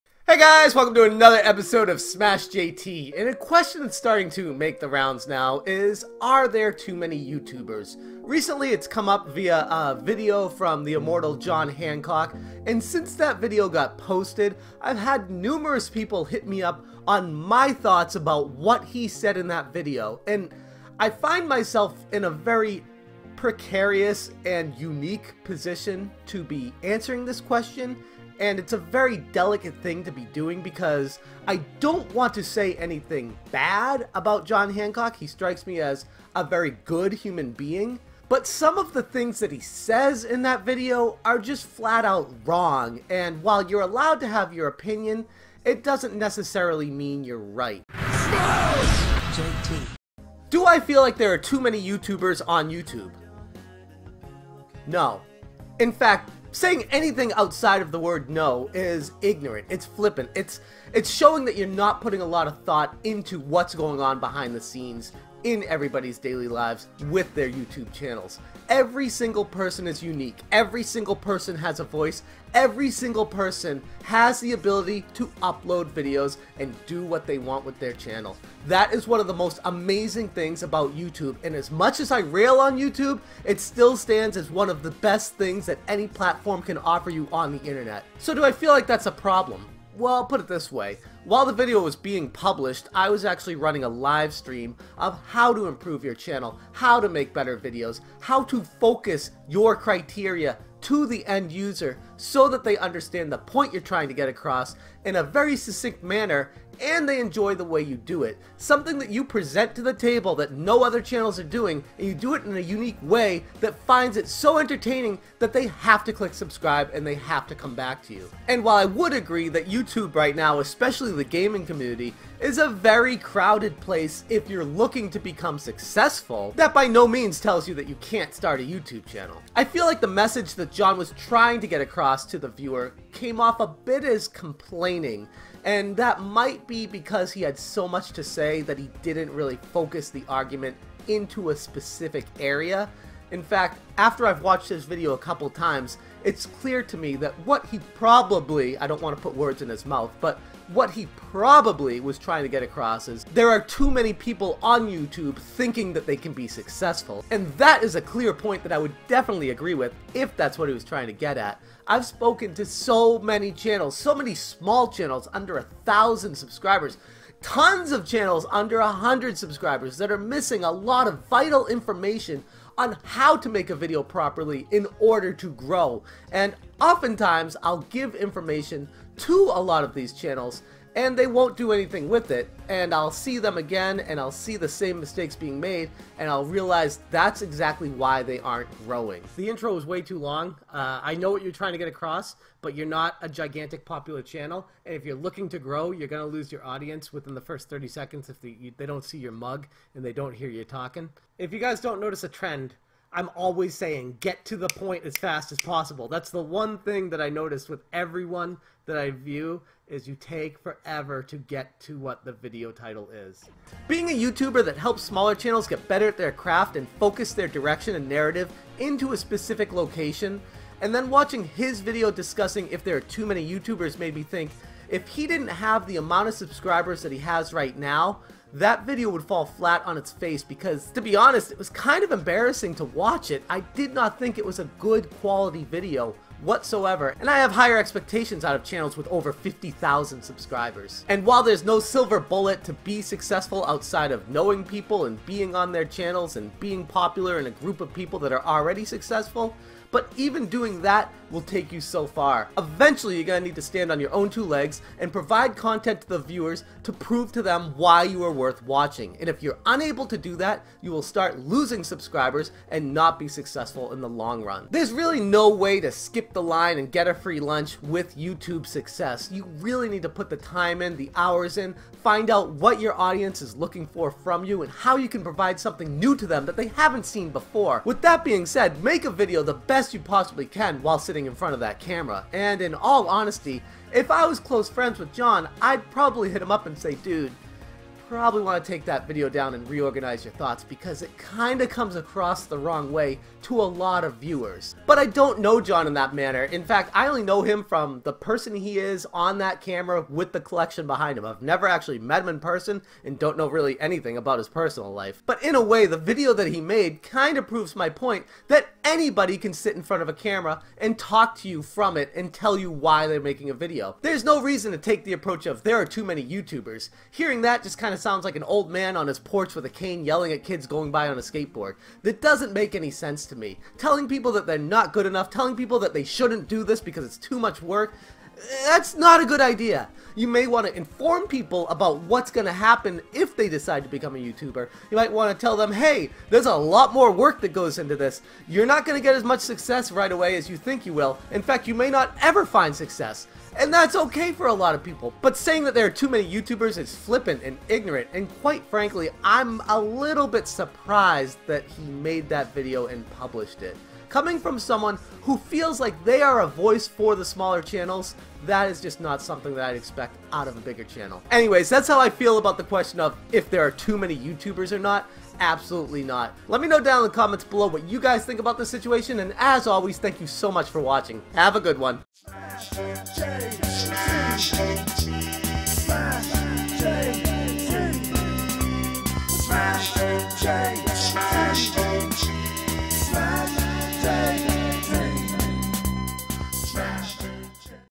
TOO MANY YouTube Channels!?
Recently, John Hancock, part of the Metal Jesus Rocks crew, released a video regarding his controversial stance on if there are too many YouTubers. Since the video was published, it has really divided fans of his channel. I’ve been contacted by multiple people asking for my thoughts regarding his opinion and what I thought about the issue in general.

Where do you stand: are there TOO MANY YouTubers? sound off in the comments!

Wanna mail something?

-JT